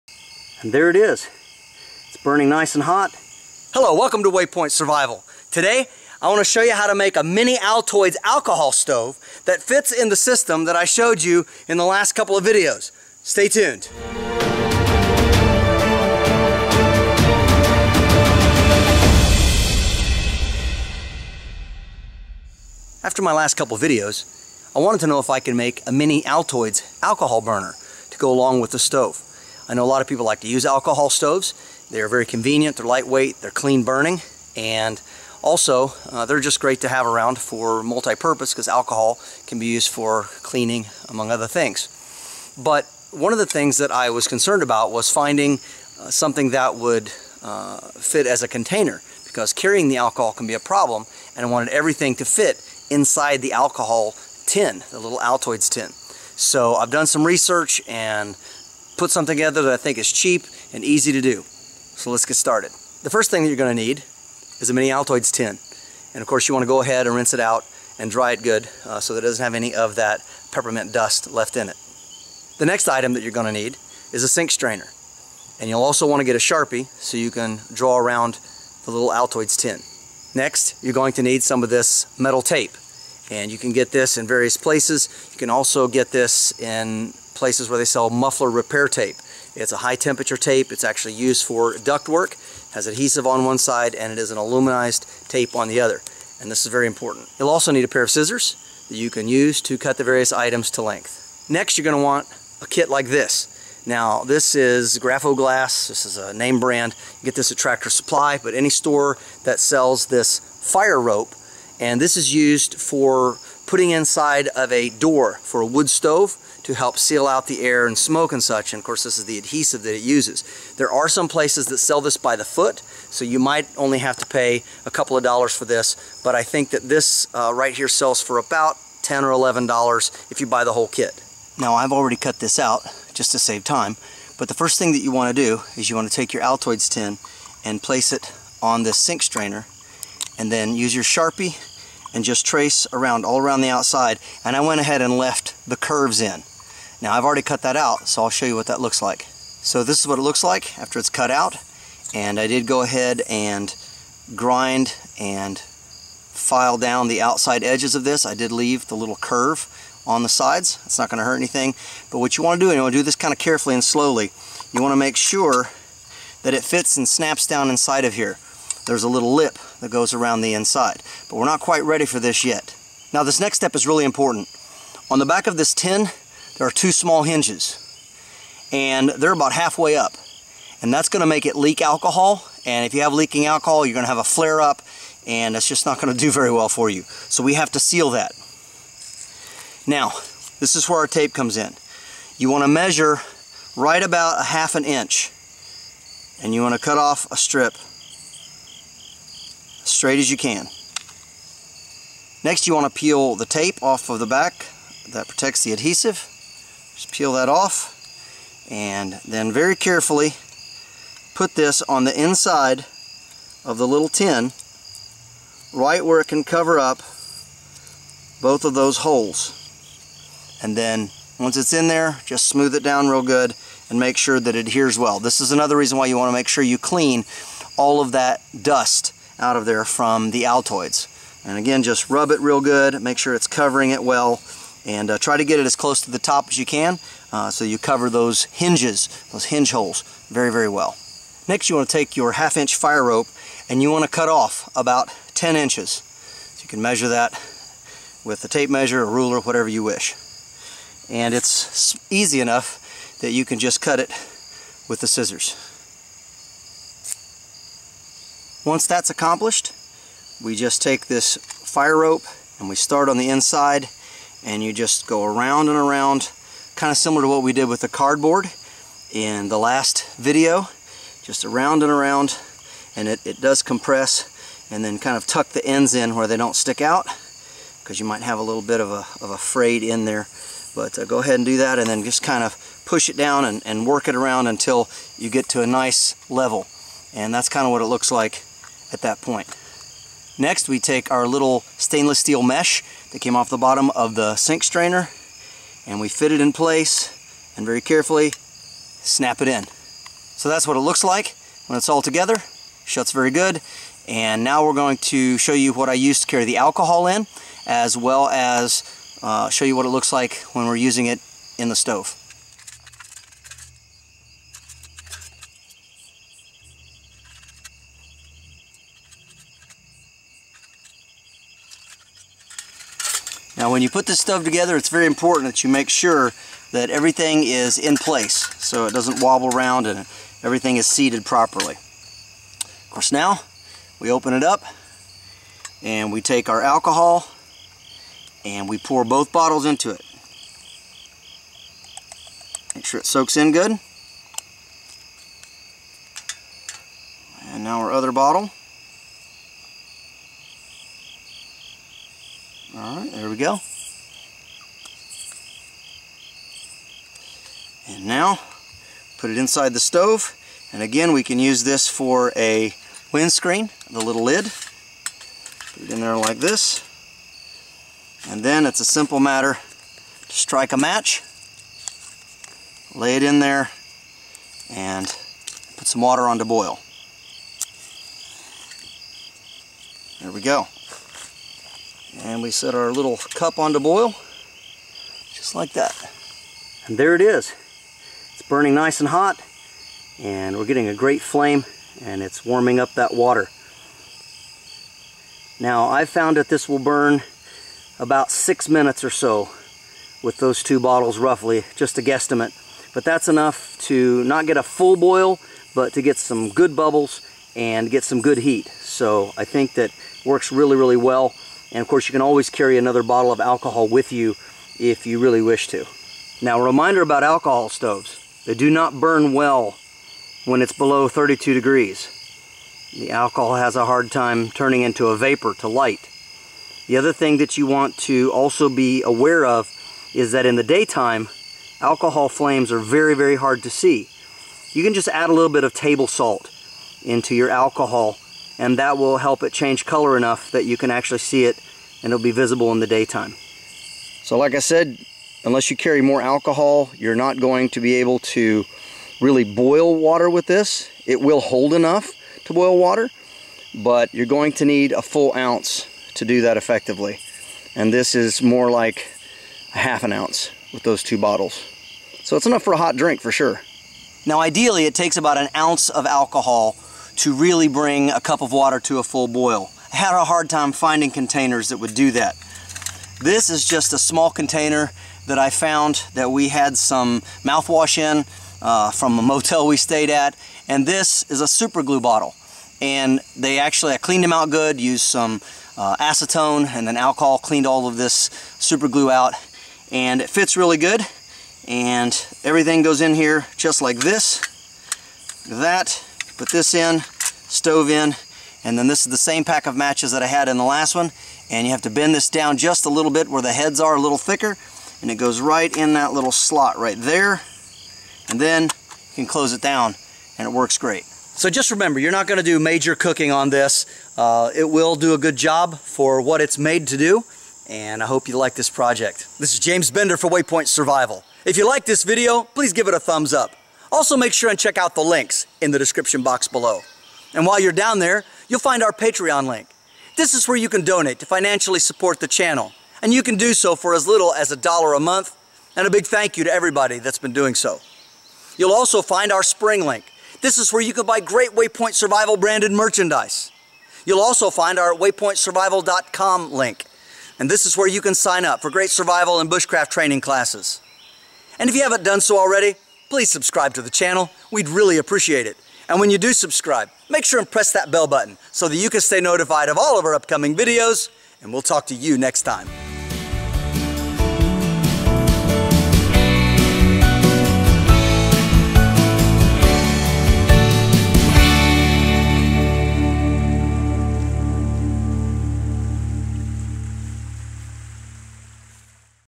Fantastic Mini Altoids Alcohol Stove! [New Design]
This fantastic mini Altoids alcohol stove is just right for that cup of coffee, tea or hot chocolate in when the temps drop! It's compact and fits right into the larger Altoids stove that I demonstrated in a previous video and even has room for it's own fuel and matches for lighting it!